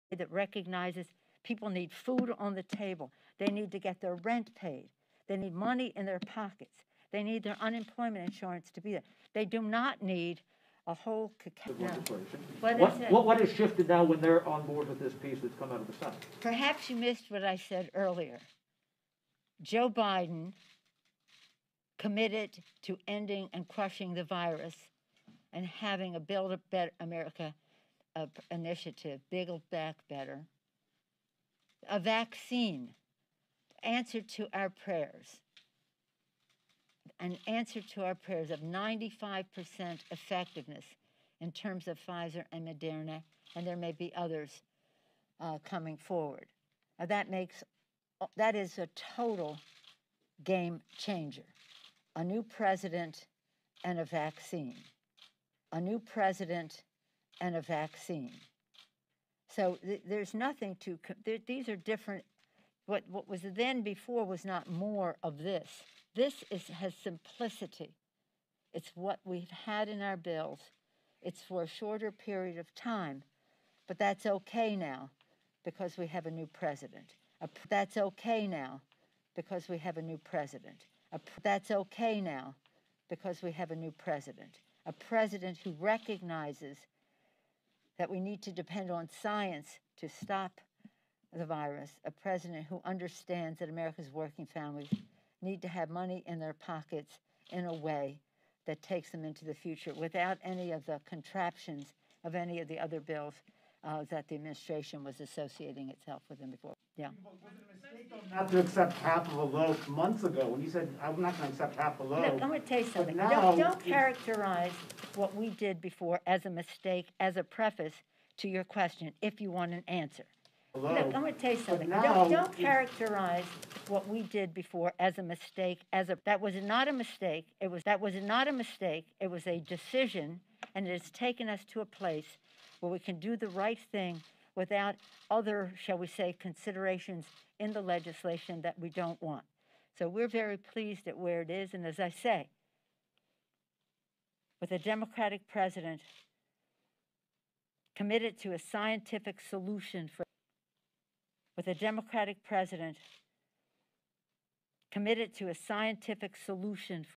PELOSI STIMULUS: I passed This Bill Because We Have A Democratic President NOW
So basically we had to wait for stimulus money because Pelosi wanted to allow Republicans to suffer until she got a democratic president.  we knew the whole time she was full of it!!..